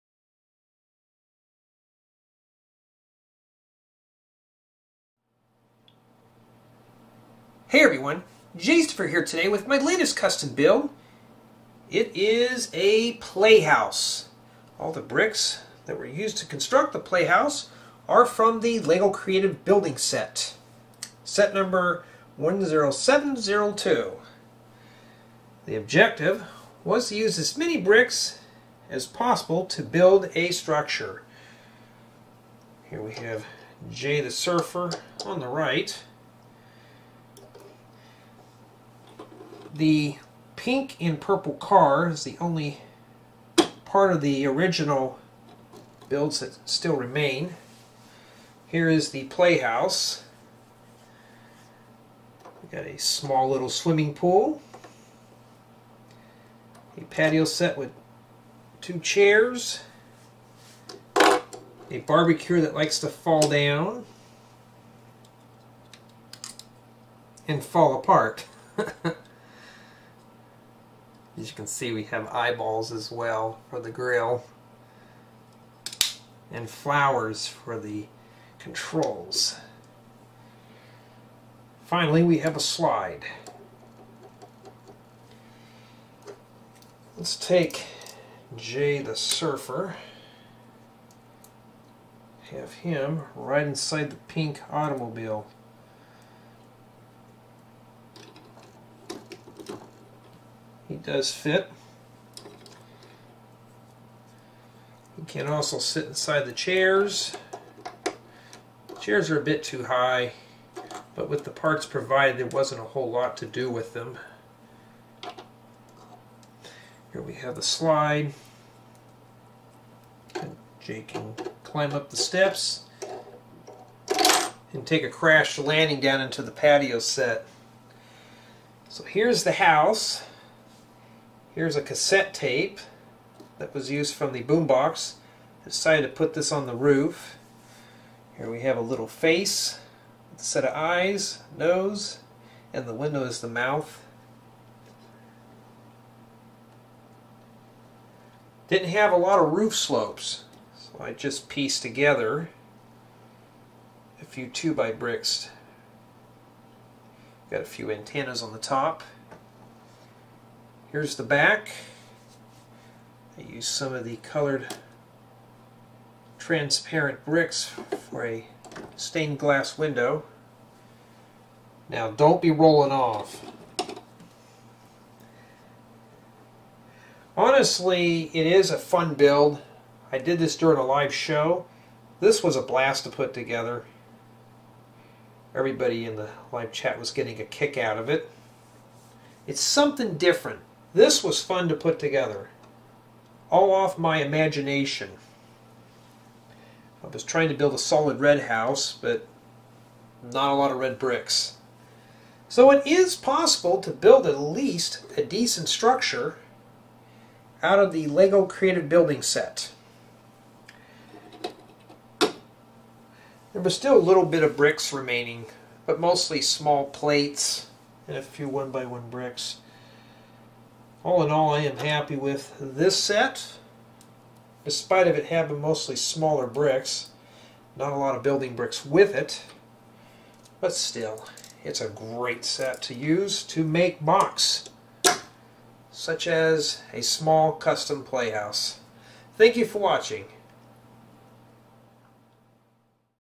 LEGO Playhouse MOC Custom Build MOC Tour
Take a tour of a custom LEGO Playhouse MOC created with bricks from Creative Building Set by LEGO Classic (10702). This is a fun colorful build that took an hour to build. Special thanks to AWM LEGO Films ) for the name of the build.

JAY is an AFOL LEGO hobbyist, LEGO builder, and LEGO collector who enjoys building with LEGO bricks in his spare time.